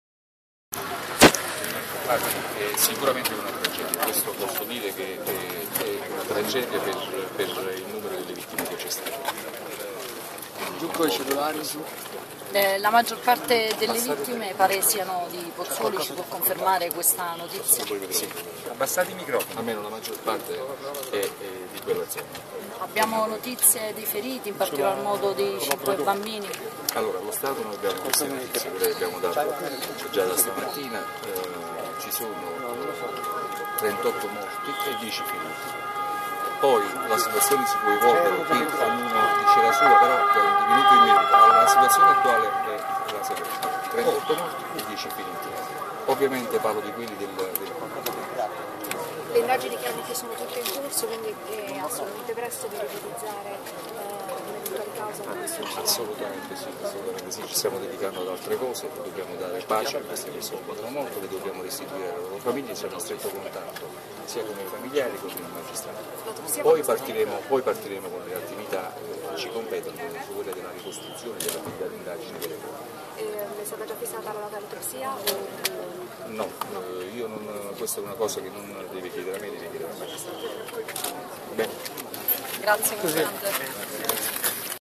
Strage A16: Un bilancio drammatico. 38 morti e 10 feriti
Salvatore Imparato, dirigente della Polizia stradale di Avellino. Tragedia, bus giù dal viadottoa Monteforte Irpino